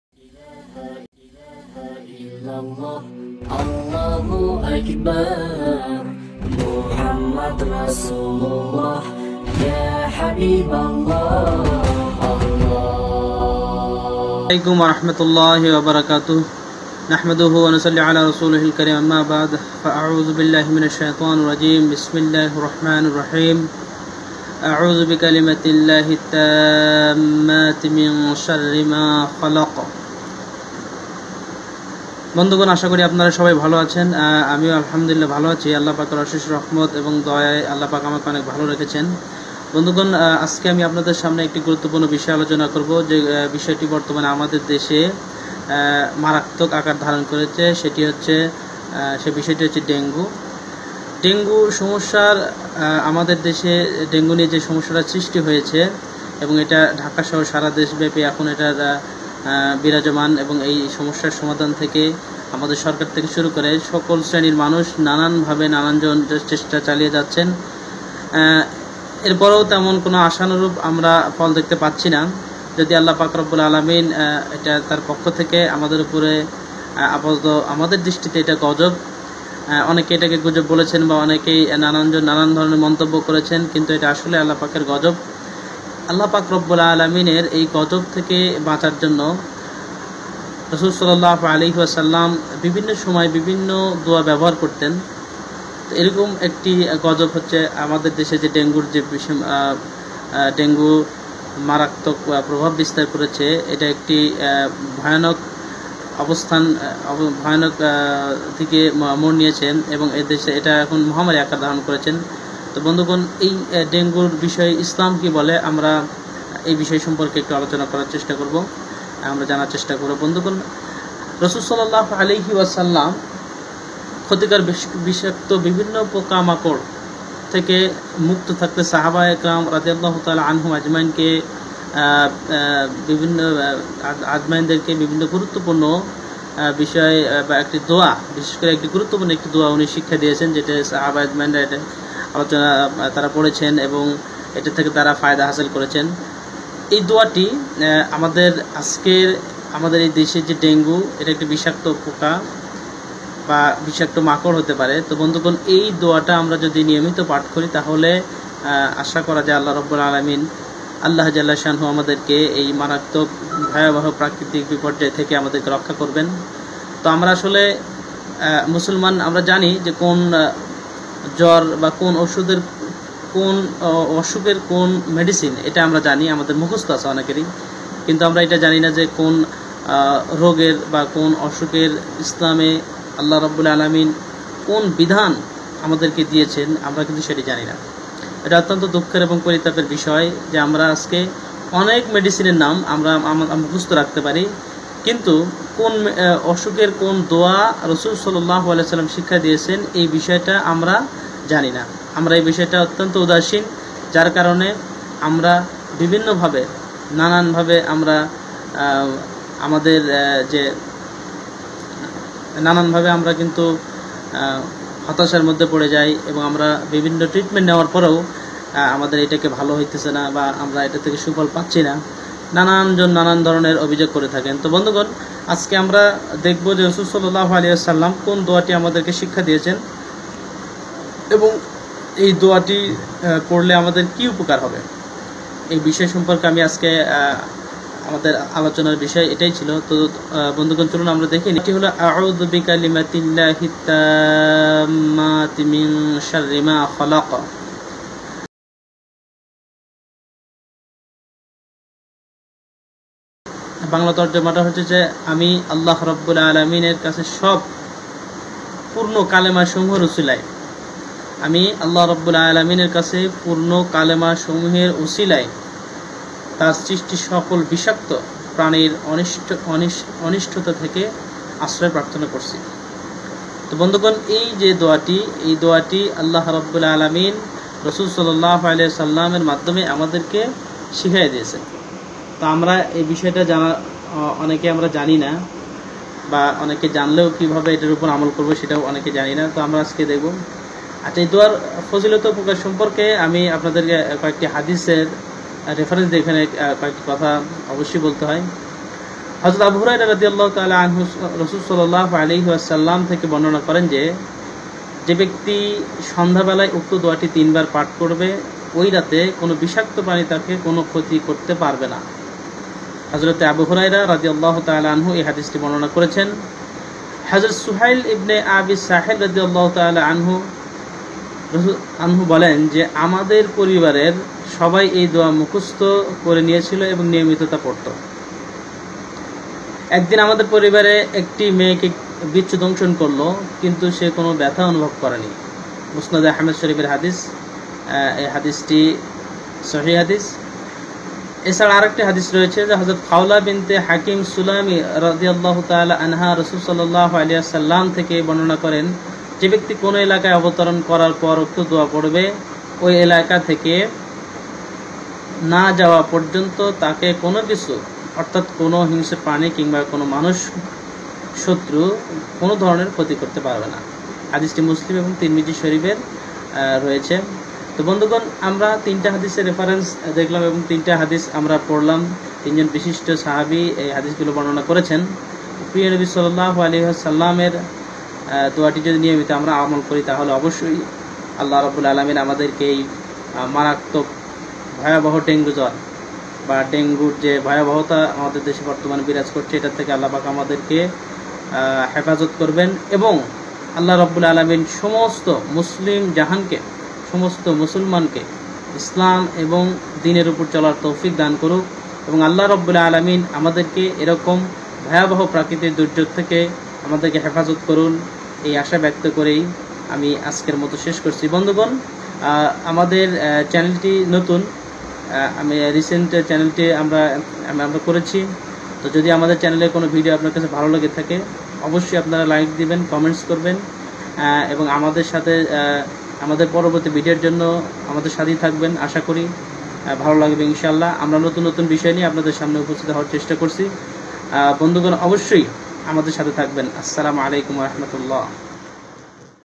ডেঙ্গু মশা থেকে বাঁচতে যে দোয়া পড়বেন
হজরত আবু হুরায়রা রাদিয়াল্লাহু আনহু বলেন, একবার এক লোককে বিচ্ছু (বিষাক্ত প্রাণী) দংশন করার কারণে লোকটি ব্যথার কারণে রাতে ঘুমাতে পারল না। সকালে রাসুলুল্লাহ সাল্লাল্লাহু আলাইহি ওয়া সাল্লামকে বিষয়টি জানালে তিনি বললেন, যদি সন্ধ্যা বেলায় এ দোয়াটি পড়তে তাহলে সকাল পর্যন্ত বিচ্ছুর (বিষাক্ত প্রাণীর) দংশন তাকে কোনো ক্ষতি করতে পারতো না।’ (মুসলিম, ইবনু মাজাহ, আবু দাউদ)

রাসুলুল্লাহ সাল্লাল্লাহু আলাইহি ওয়া সাল্লাম শেখানো দোয়াটি হলো-
أَعُوْذُ بِكَلِمَاتِ اللهِ التَّامَّاتِ مِنْ شَرِّ مَا خَلَقَ
উচ্চারণ : আউজু বিকালিমাতিল্লাহিত তাম্মাতি মিং শাররি মা খালাক্বা।
অর্থ : আমি আল্লাহর সব পূর্ণ কালেমাসমূহের উসিলায়, তাঁর সৃষ্ট সব (বিষাক্ত) প্রাণীর অনিষ্টতা থেকে আমি আশ্রয় প্রার্থনা করছি।

এ দোয়ার ফজিলত ও উপকারিতা সম্পর্কে অন্য হাদিসে এসেছে-
- হজরত আবু হুরাইরা রাদিয়াল্লাহু আনহু রাসুলুল্লাহ সাল্লাল্লাহু আলাইহি ওয়া সাল্লাম থেকে বর্ণনা করেন, যে ব্যক্তি সন্ধ্যা বেলায় উক্ত দোয়াটি ৩ বার পড়বে, ওই রাতে কোনো বিষাক্ত প্রাণীর বিষ তাকে কোনো ক্ষতি করতে পারবে না।’

- হজরত সুহাইল ইবনে আবি সালেহ রাদিয়াল্লাহু আনহু বলেন, আমাদের পরিবারের সবাই এ দোয়াটি মুখস্ত করে নিয়েছিল এবং নিয়মিত তা পড়তো, একদিন আমাদের পরিবারের একটি মেয়েকে বিচ্ছু দংশন করলো কিন্তু সে কোনো ব্যথাই অনুভব করেনি। (মুসনাদে আহমদ)

- হজরত খাউলা বিনতে হাকিম সুলামি রাদিয়াল্লাহু আনহা রাসুলুল্লাহ সাল্লাল্লাহু আলাইহি ওয়া সাল্লাম থেকে বর্ণনা করেন, যে ব্যক্তি কোনো এলাকায় অবতরণ করার পর উক্ত দোয়াটি পড়বে ওই এলাকা থেকে না যাওয়া পর্যন্ত তাকে কোনো কিছু (কোনো হিংস্র প্রাণী কিংবা মানুষ শত্রু) কোনো ধরণের ক্ষতি করতে পারবে না।’ (মুসলিম, তিরমিজি)